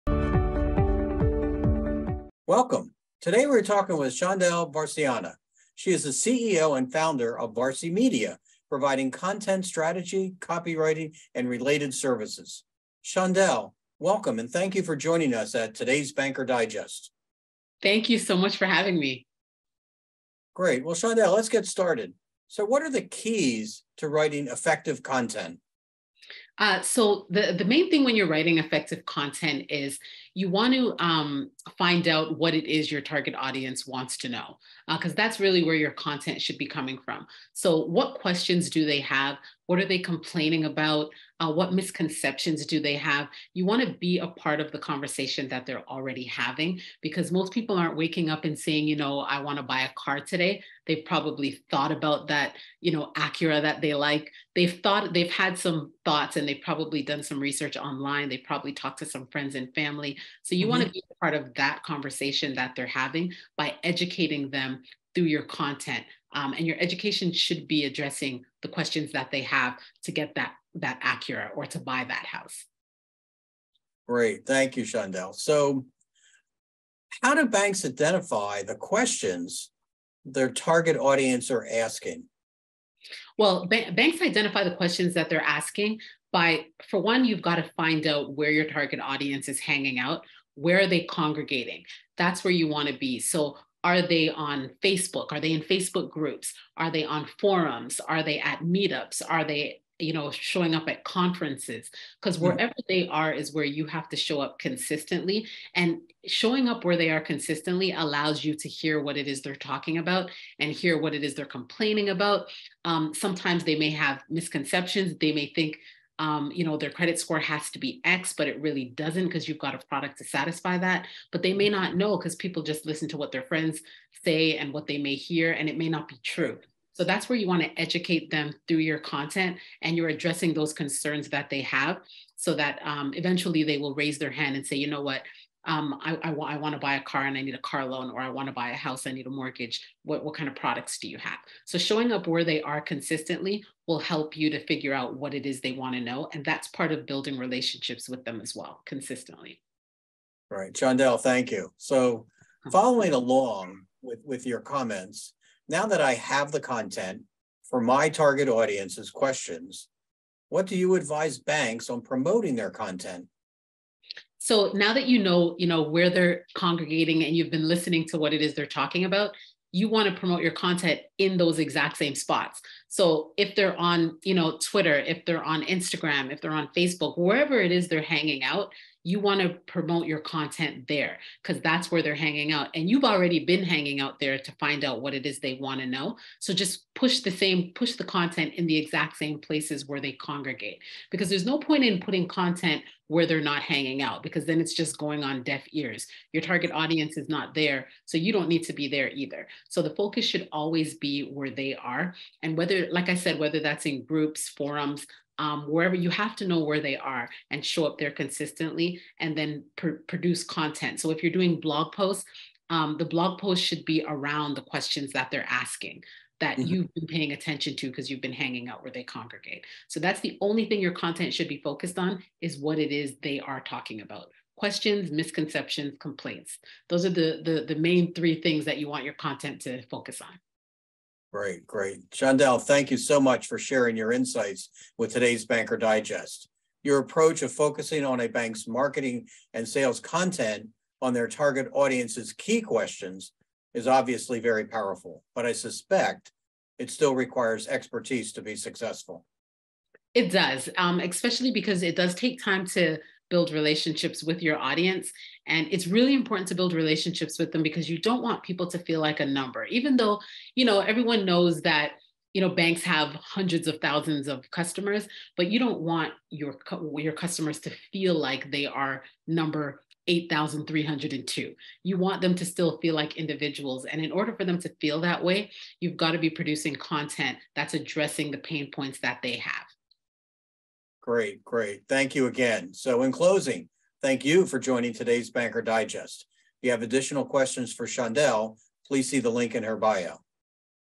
Keys to Creating Effective Content
Shondell Varcianna, CEO and Founder of Varci Media, discusses the keys to creating effective content that resonates with your target audiences.